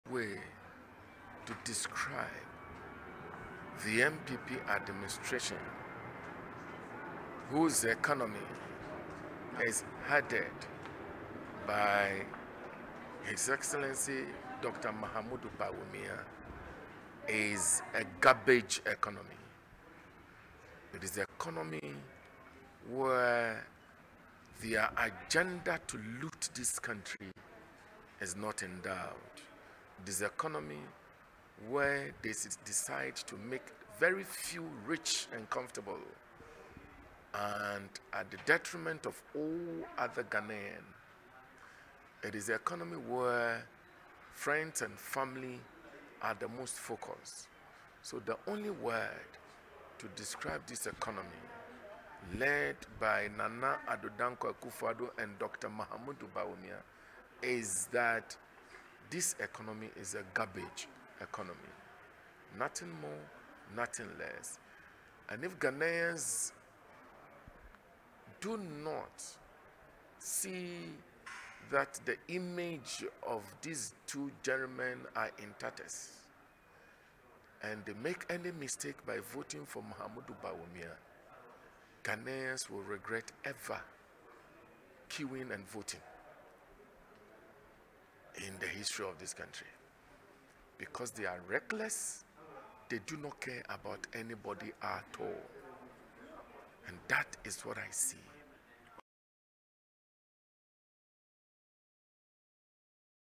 THE NPP ECONOMY IS A GARBAGE ECONOMY - DAVID T. VONDEE
The member of parliament for Twifo Atti Morkwa constituency, Hon. David T. Vondee has stated that the New Patriotic Party led by president Akuffo-Addo and Bawumia is a garbage economy and nothing more or less than that.

He added that this government is reckless hence they do not care about Ghanaians therefore the National Democratic Congress flag bearer,  John Mahama is the best choice for Ghanaians so every Ghanaian should not make a mistake by voting for the NPP in the general election.